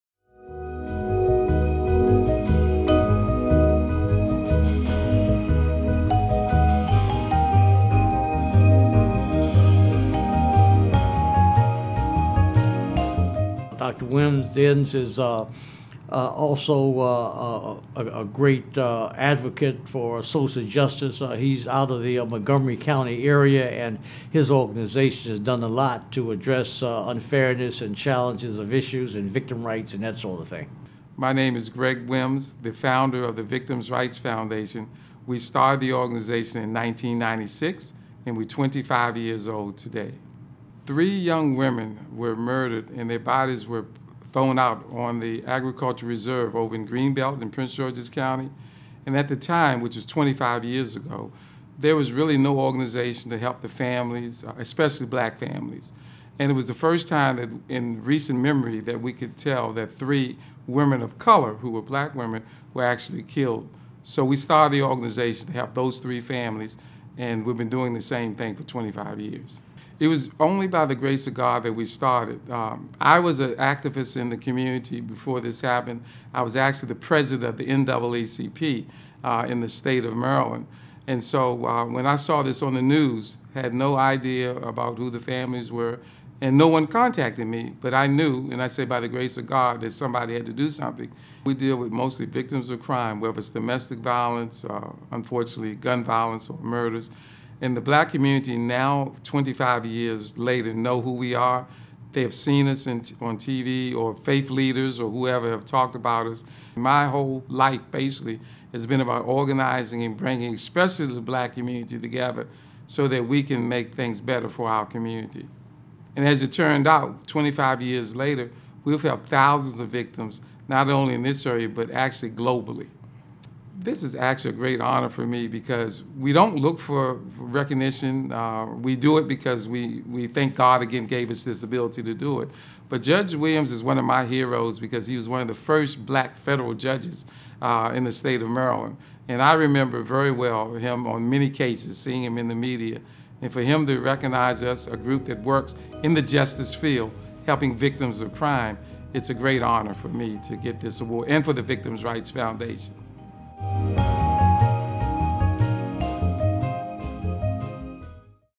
2021 Judge AW Lecture Honoree & JUSTICE Award Recipient, The Victim's Rights Foundation
Gregory Wims is a local businessman and community activist who founded the Victims' Rights Foundation (VRF) in response to the senseless killings of three Washington D.C. area women in 1996. Moved by the grace of God, Gregory is dedicated in supporting victims and their families of violent crimes and attacks. He was instrumental in forming and supporting the Sniper Victims’ Fund in response to the sniper attacks in the Washington D.C. metro area in 2002. He raised $500,000 for the victims families.

In the grand scheme, however, he is really a man with a long volunteer career and a burning desire to bring comfort and aid to those in need. In the last 43 years he has raised more than 2 million dollars, logged more than 1 million miles and volunteered more than 42,000 hours on community service projects.

He began his volunteer career in 1969, when he was elected vice president of the State of Maryland Youth Commission. The next year, as president of that organization, he played a pivotal role in lowering the voting age from 21 to 18. In the 1970’s, he served as the youngest person ever appointed by the County Executive as Commissioner of the Montgomery County Maryland Human Relations Commission. In this role, he worked with community officials to hire the first African American to the police department.

From 1974 to 1976, he was the first male Head Start teacher in Montgomery County, Maryland. In the 1980s, he led the Social Concern Committee at Goshen United Methodist Church and set up a prison ministry program. He also founded United Brothers, Inc., organized the first “gun turn-in” program in Montgomery County, and chaired the “Get Out to Vote” campaign.

In the early 1990s, Gregory Wims served as the membership chairman for the NAACP, recruiting more than 1,000 new members. In 1994, he was elected president of the Montgomery County Chapter of the NAACP, and one-year later; he was voted President of the NAACP’s Maryland State Chapter. In this role, he led more peaceful demonstrations than any previous president and organized with Janice Washington the first federal employment task force for the National NAACP. During this, he was also appointed by President George Bush as a Board Member of the Kennedy Center for the Performing Arts in Washington, DC.

Mr. Wims currently serves on the following boards: Universities of Maryland at Shady Grove, University of Maryland Dental School, Montgomery College Foundation Board, Boys and Girls Clubs, YMCA, Children"s Charities Foundation, Bethesda Chevy Chase Rotary Club Foundation Board. He is currently serving as Area Governor for the Bethesda Clubs in District 7620 for Rotary International. He is also the Producer and Host for "Montgomery Matters" a local TV Show seen on Channel 21, Montgomery Community Media.